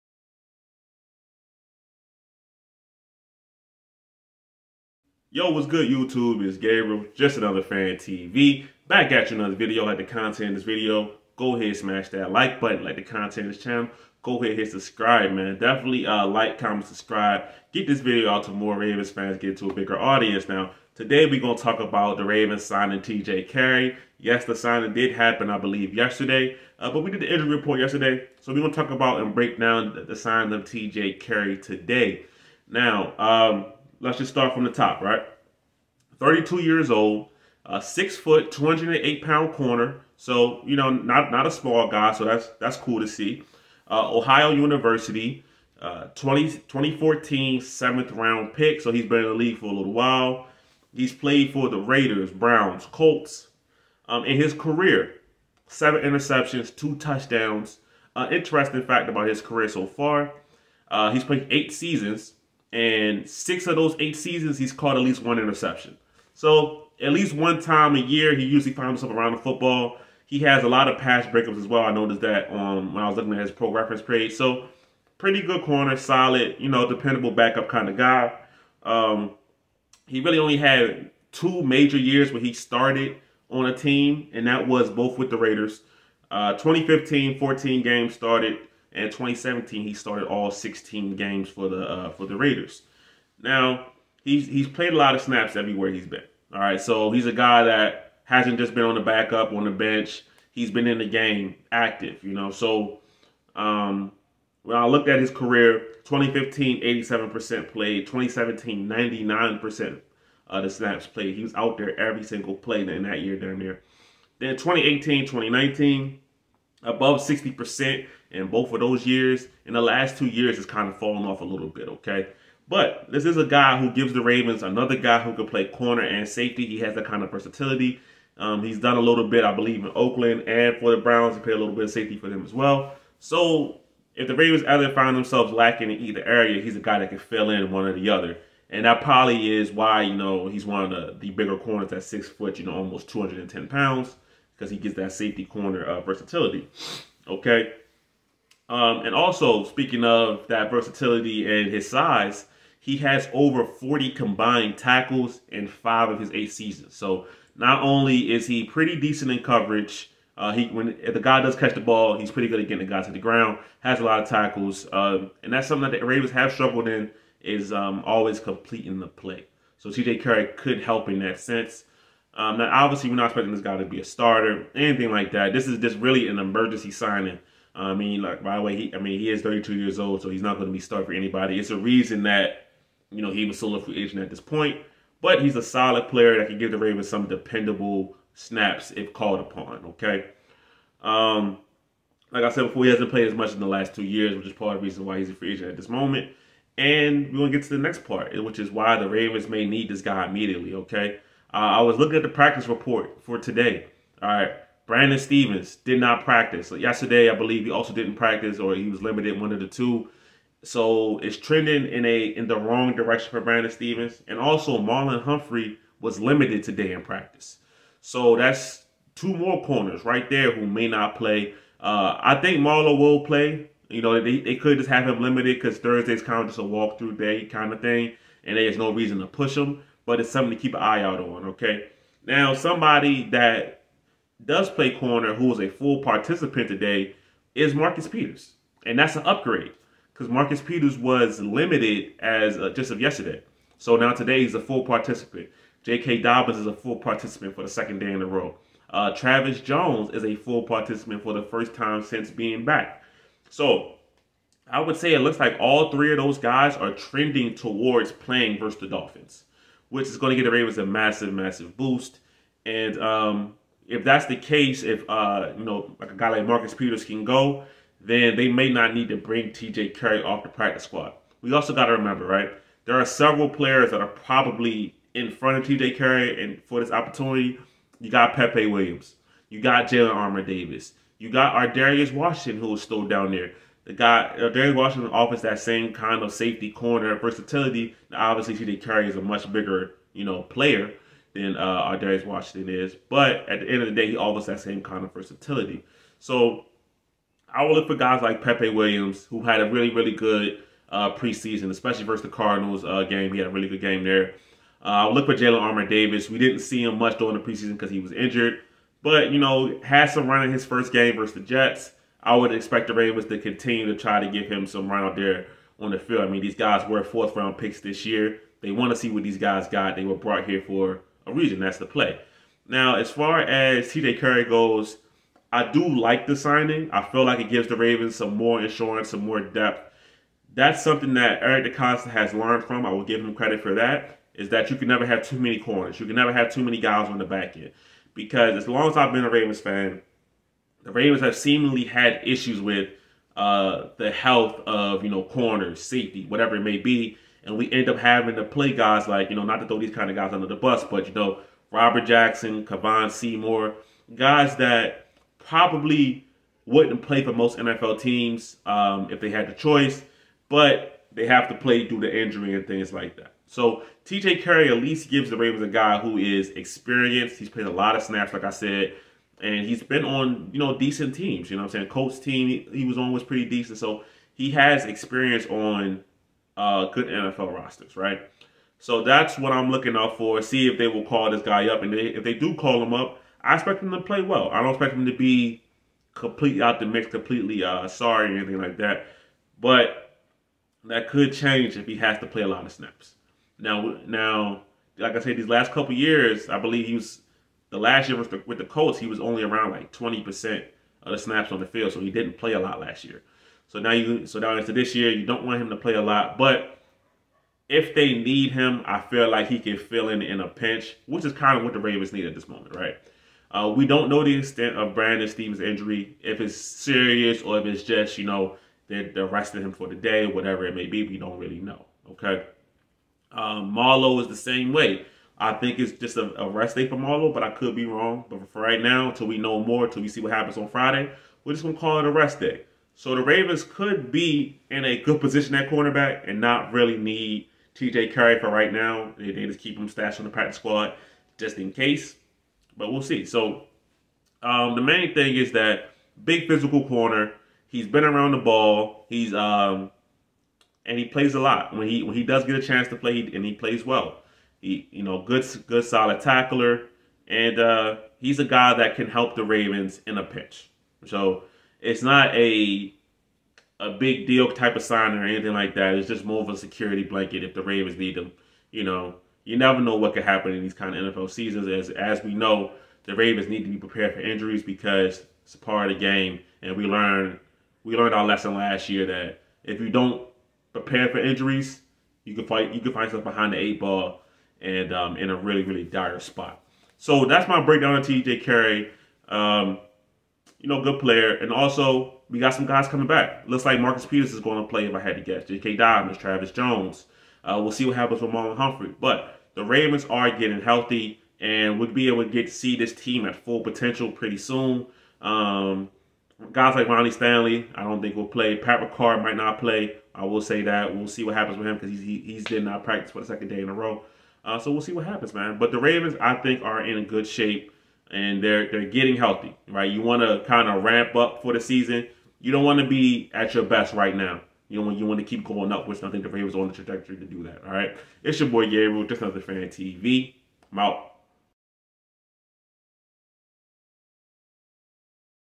Ravens Sign TJ Carrie & He Might Be Needed Immediately (Breakdown of the Singing)
Ravens Sign TJ Carrie and after some of the injury news from today's practice, he may need to play right away!